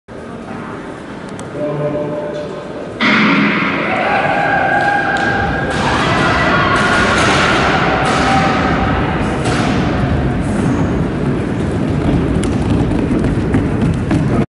II Halowy Sprawdzian PZLA - 60 m ppł - U20 - K - 2/2
1 JÓŹWIACZUK Katarzyna 8.83
2 KŁOS Zuzanna 9.22
3 FILAR Gabriela 9.23
4 GAJEWSKA Karolina  9.36
5 ŚNIADKOWSKA Gabriela 9.81
 NOWAK Zuzanna 01-05-22 DNF